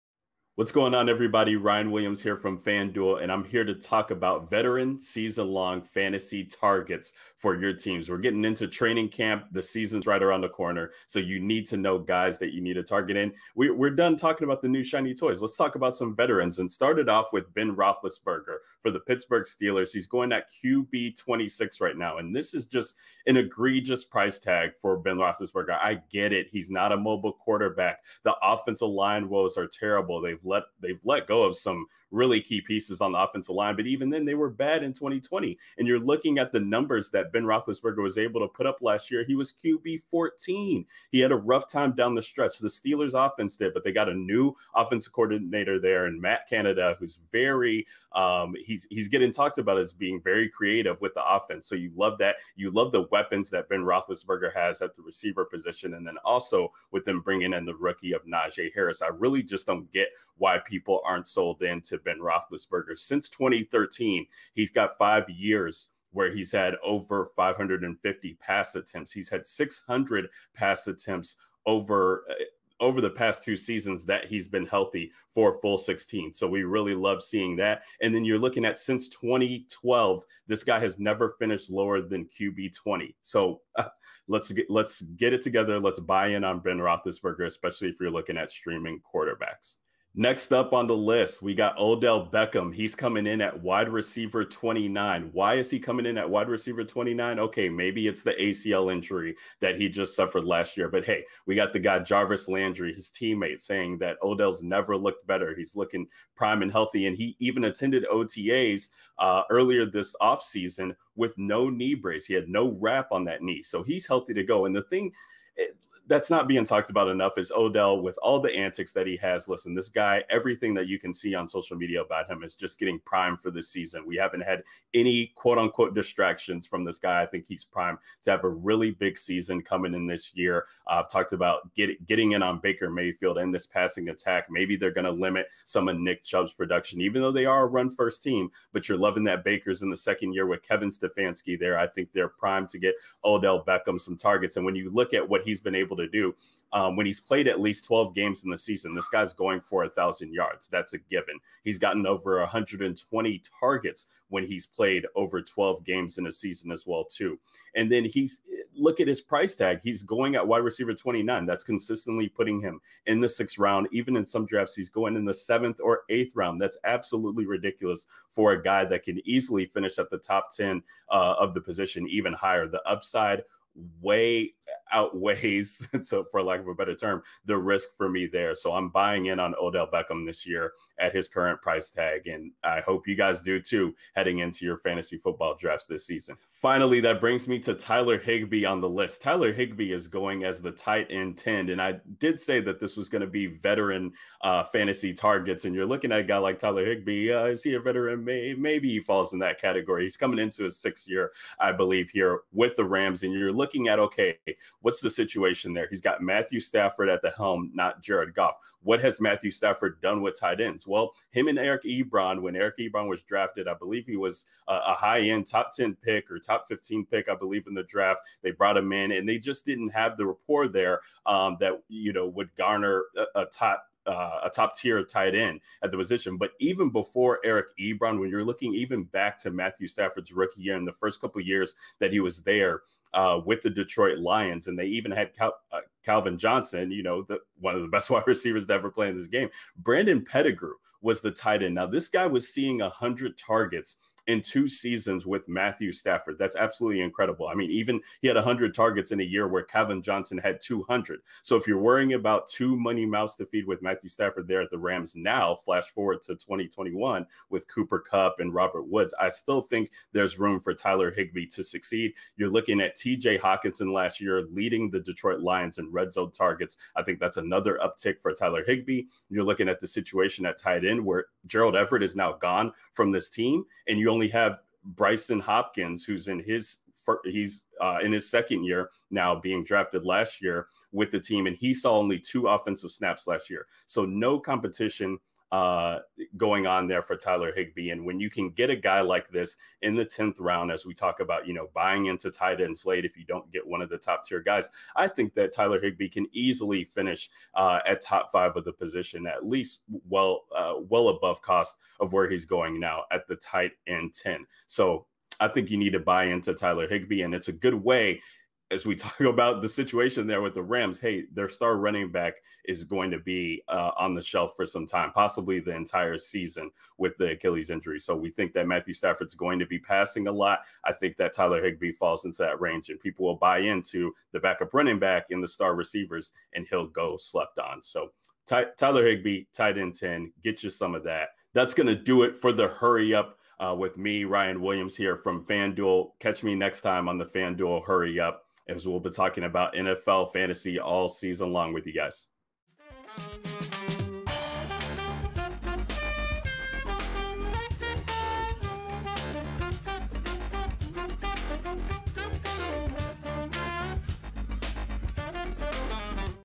2021 Season-Long Fantasy Football Veteran Targets | Hurry Up
2021 Season-Long Fantasy Football veteran options for you to pick from including a Super Bowl MVP, a star coming back from injury, and a possible sleeper tight end. Ryan Williams navigates some of the NFL veteran options for your 2021 season-long fantasy football leagues on the latest episode of 'The Hurry Up'.

0:00 Intro
0:18 Big Ben
1:27 Odell Beckham Jr
3:07 Tyler Higbee